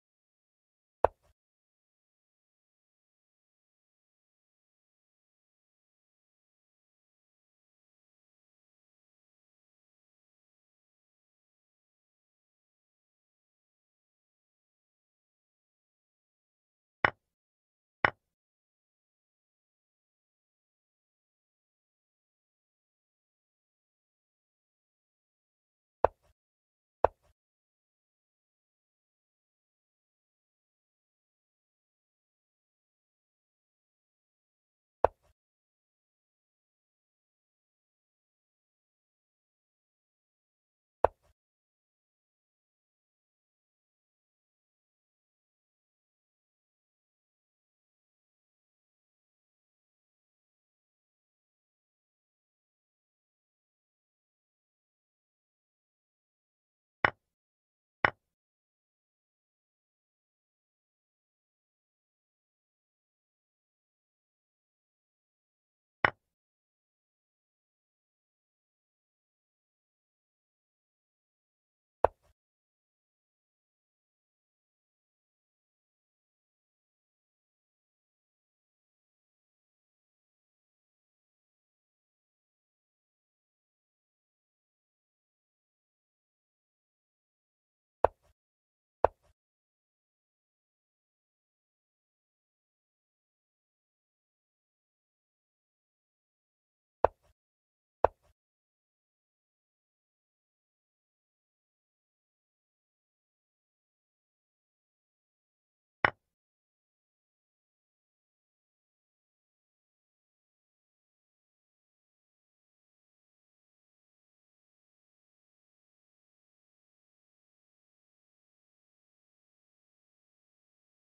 Chess Puzzles to Practice. Themes: Crushing, Endgame, Pawn endgame. Learn Chess
✅ PUZZLE THEMES
Puzzle 1
• Pawn endgame: An endgame with only pawns.
Puzzle 2
• Short puzzle: Two moves to win.
Puzzle 3
• Interference: Moving a piece between two opponent pieces to leave one or both opponent pieces undefended, such as a knight on a defended square between two rooks.